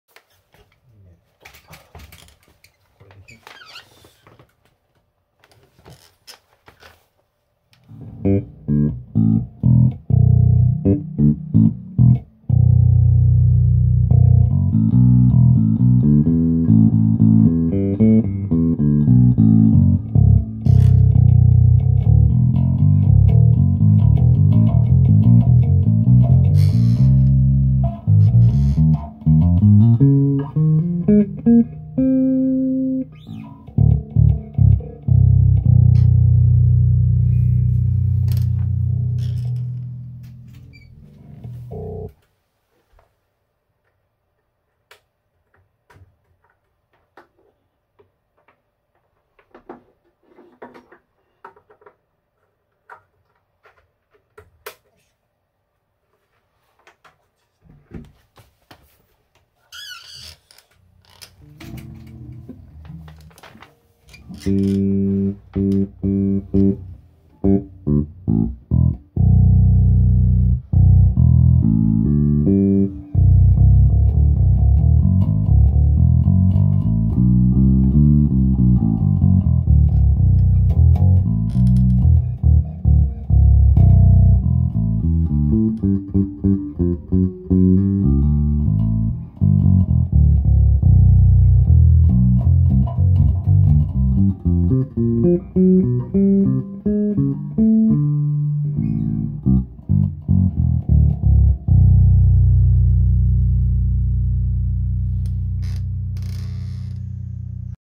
Trickfish BULLHEAD MINI500 and TF112,TF112M DEMO
話題のUSAトリックフィッシュ。

新機種MINI500を使って同じ１２インチの
TF112とTF112Mを鳴らし比べてみました。

　九州のベース専門店
　TiesStore タイズストア
　福岡県北九州市小倉北区浅野３－８－１
　♪店頭展示有、試奏OK(ご予約制）♪
　♪お得なポイント還元あり！♪
　♪「タイズストア」で検索♪